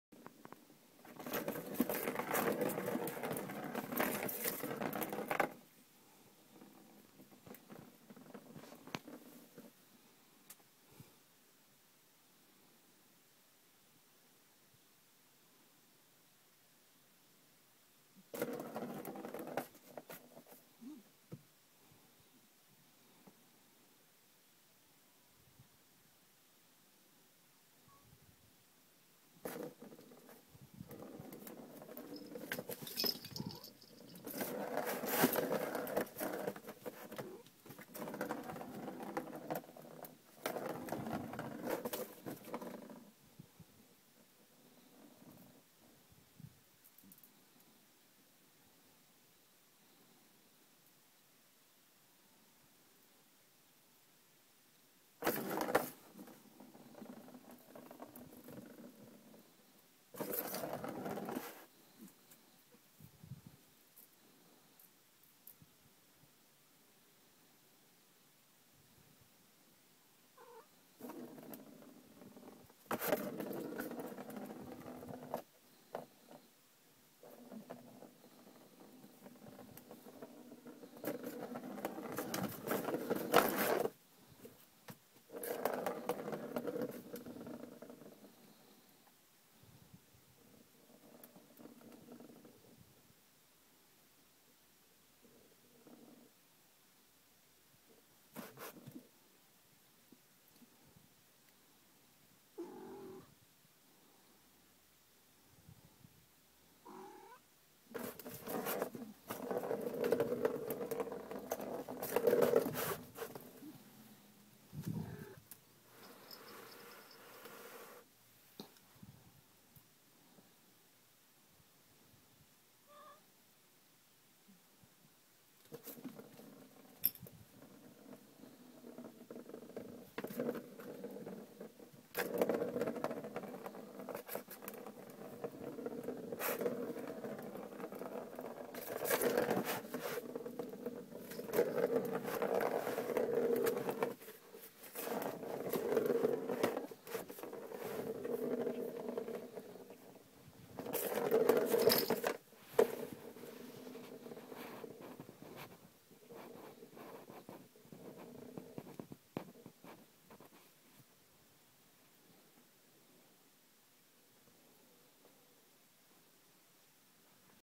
Danny Cat Playing with Circle Ball
Danny playing with circle ball toy